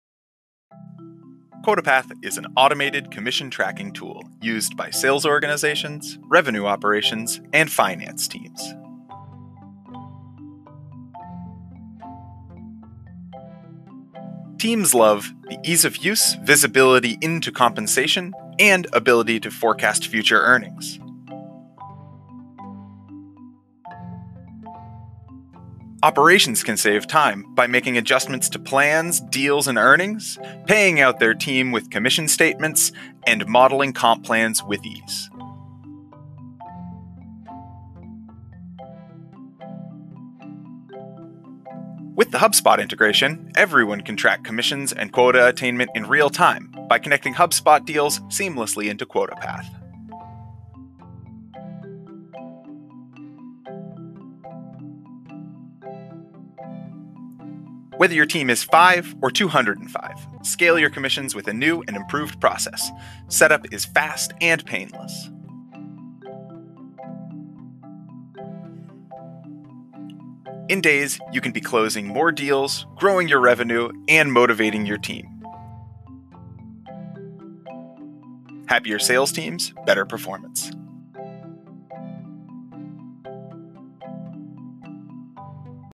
Automate commissions with QuotaPath & HubSpot Sales CRM
QuotaPath has integrated with HubSpot! With this CRM integration, sales reps can track quota attainment & earnings in real-time.

Import HubSpot deals to automate the commission process. Close more deals, grow your revenue, and motivate your team.